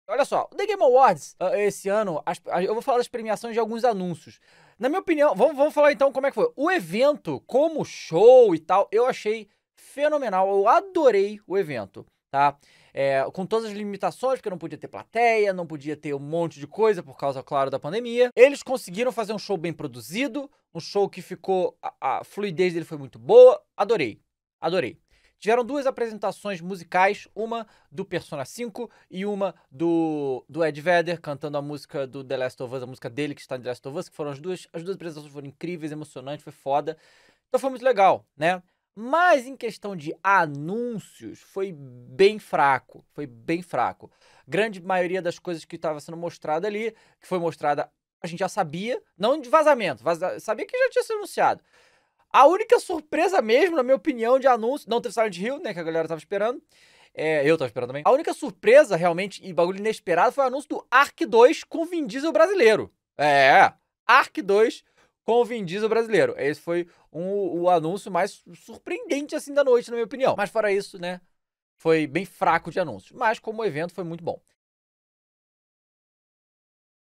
Me DECEPCIONEI com esse evento?
Fala galera! No corte de hoje falaremos do The Game Awards e tudo que Davy achou do evento do oscar dos jogos do ano.